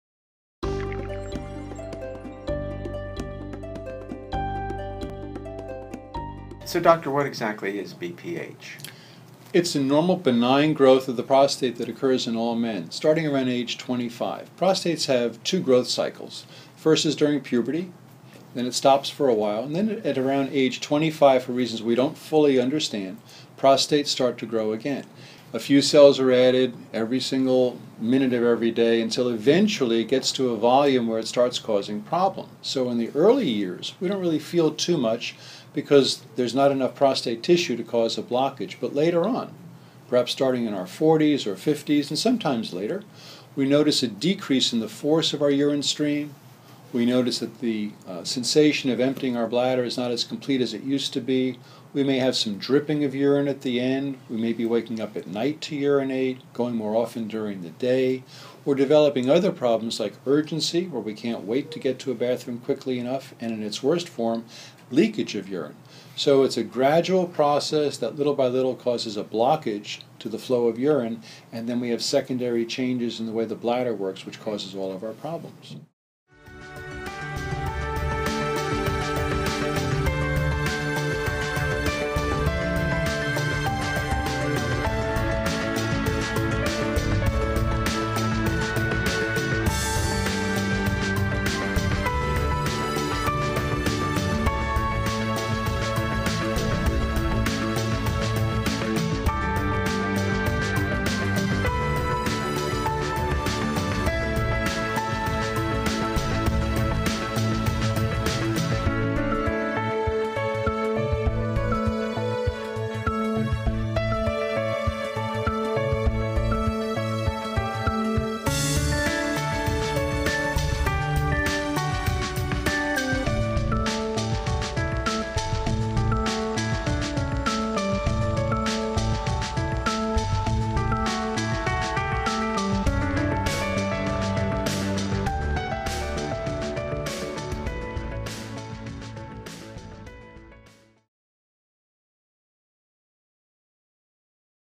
What is Benign Prostatic Hyperplasia (BPH)?
BPH is a normal, common and benign growth of the prostate that occurs as men age. As the prostate grows it can begin to alter normal urinary patterns. Many men may feel a decrease in urinary flow, feel like they are not emptying their bladder fully, wake up at night to urinate, have a sense of urgency and/or to go to the bathroom amongst other symptoms.